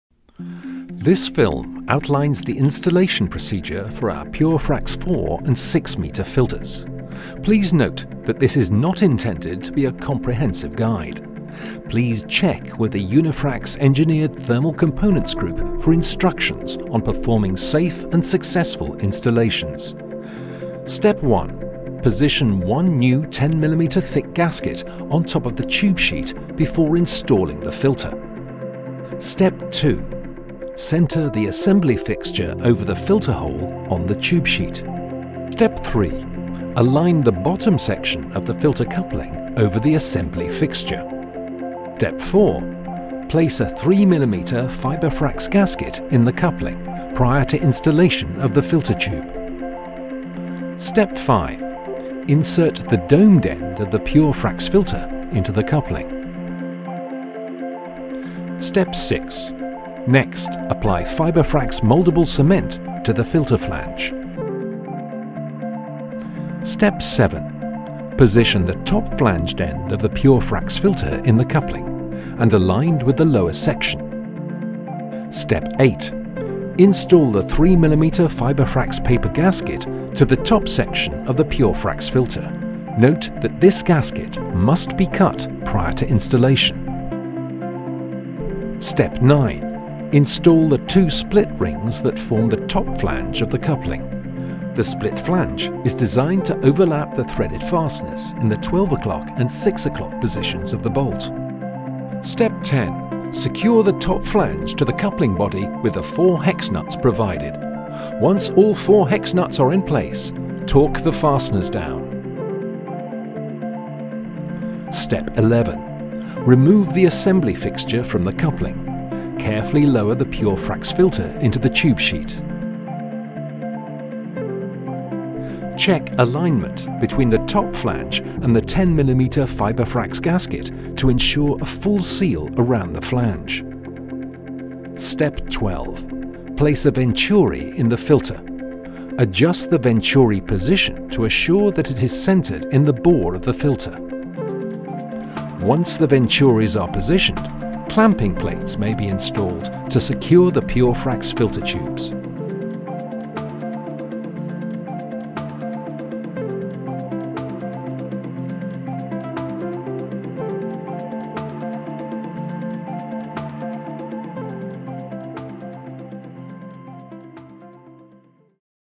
Purefrax Step by Step Video March 2018
Fiberfrax® Purefrax® Hot Gas Filters

Purefrax® filter elements are vacuum formed using Unifrax’s refractory ceramic fibers (Fiberfrax®) made into a slurry, together with organic and inorganic binders. The unique binding system means that low levels of organic binder are present so that the Purefrax filters retain strength and integrity when exposed to heat.Description